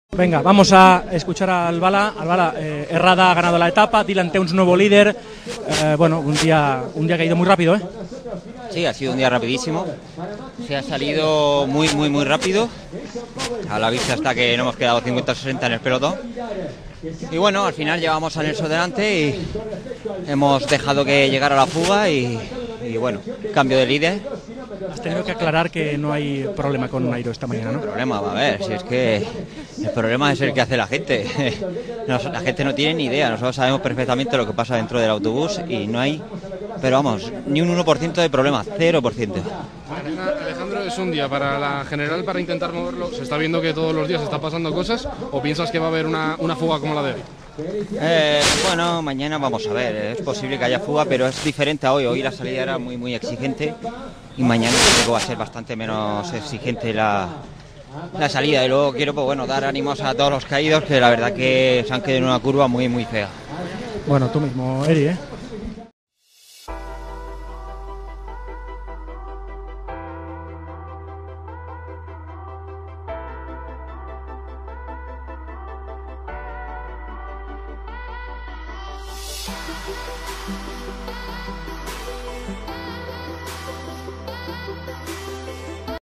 Alejandro Valverde ¿ENFADADO CON NAIRO? Declaraciones Etapa 6 Vuelta a España 2019
Alejandro Valverde en declaraciones tras finalitar la etapa 6 de la Vuelta a España 2019, dónde habla sobre su relación actual con su aún compañero de equipo, Nairo Quintana.